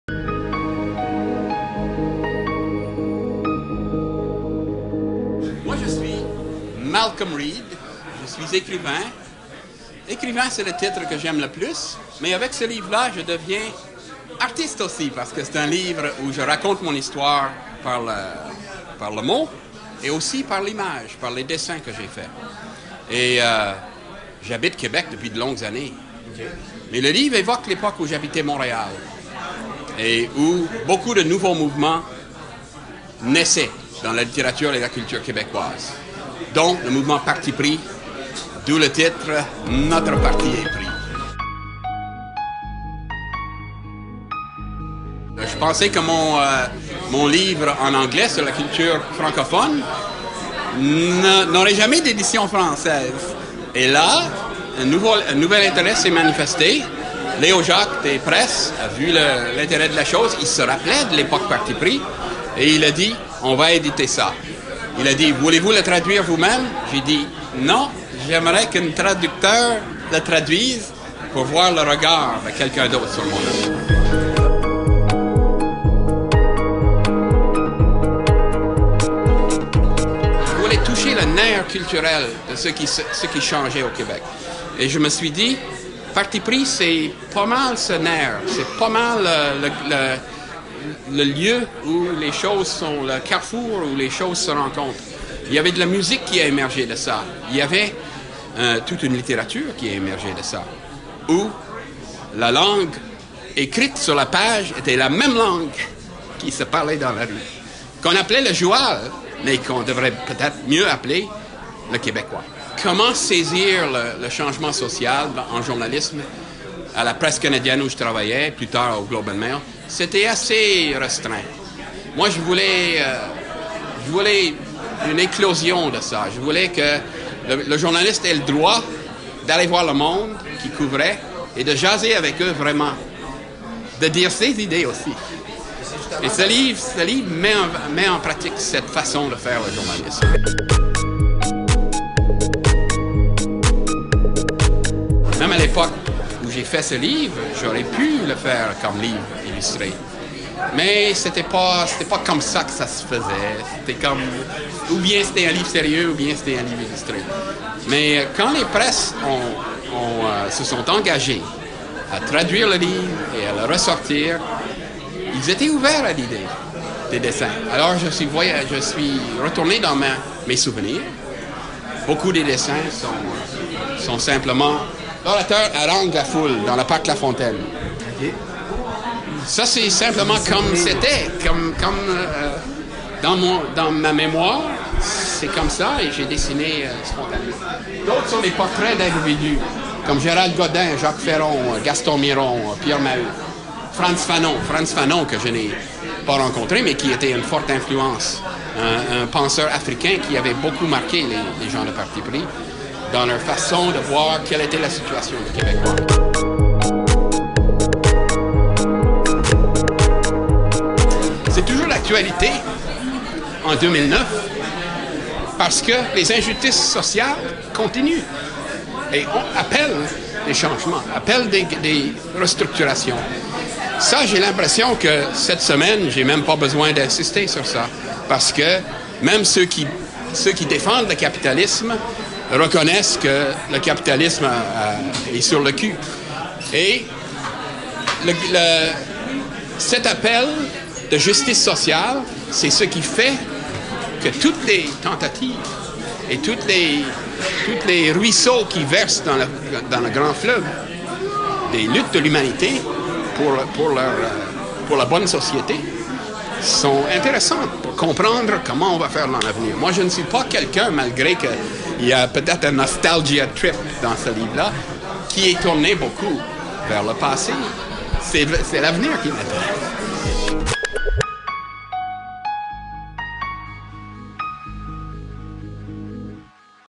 Notre parti est pris. Un jeune reporter chez les écrivains révolutionnaires du Québec, 1963-197
Écrit entre 1966 et 1970 et publié à New York et à Toronto en 1972, The Shouting Signpainters, A Literary and Political Account of Quebec Revolutionary Nationalism trouve son public naturel près de quarante ans plus tard.

Pour faire la chronique de la mouvance politique et littéraire du Québec des années 1960, Malcolm Reid, journaliste canadien-anglais alors dans la vingtaine, rencontre les météores de Parti pris. Jacques Renaud, auteur du premier roman en joual, Le Cassé. Paul Chamberland et son épique Lafficheur hurle. André Major, pour qui la fiction se puise « la semaine dernière pas loin du pont ». Et dautres encore, partipristes, précurseurs ou successeurs : Pierre Maheu, Gérald Godin, Gaston Miron, Jacques Ferron, Raôul Duguay, Clémence Desrochers, Claude Dubois

Plus quun essai-reportage, ce livre est un chant de solidarité avec les jeunes intellectuels de gauche qui militaient pour un Québec indépendant, socialiste et laïque. Entre les extraits de textes phares, les analyses politico-littéraires, les entretiens avec les auteurs, Malcolm Reid, dans un style libre et enthousiaste, glisse des anec dotes et esquisse des portraits.

Dans Notre parti est pris, il revisite son texte en y ajoutant sa touche de dessinateur, en guise dhommage aux penseurs et aux poètes rebelles qui depuis ont marqué lhistoire.